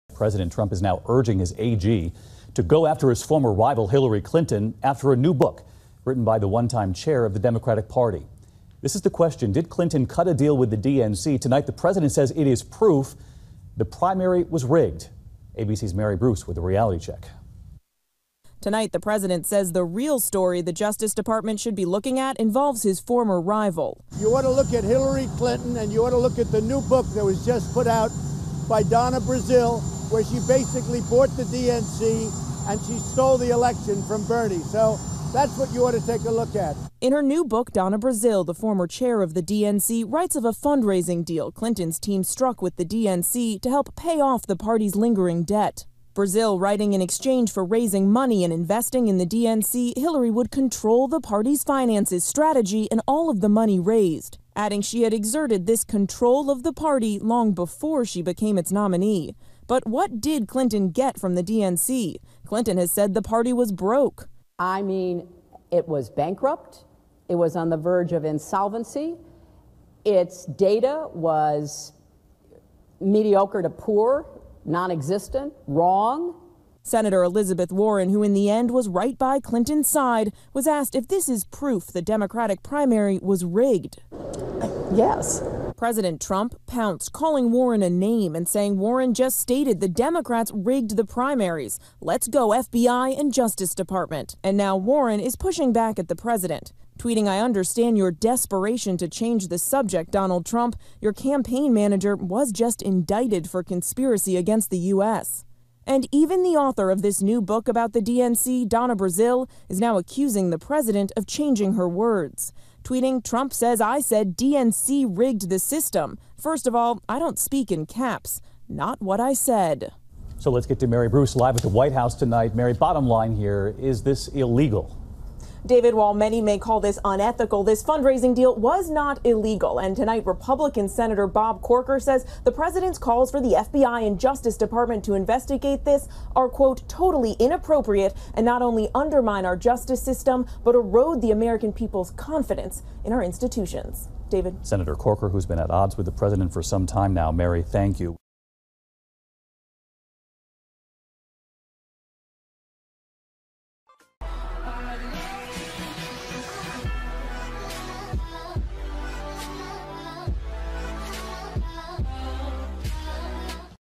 Trump pushes for investigation of Hillary Clinton and DNC - NEWS TODAY
The president leaves today for a 13-day trip to Asia.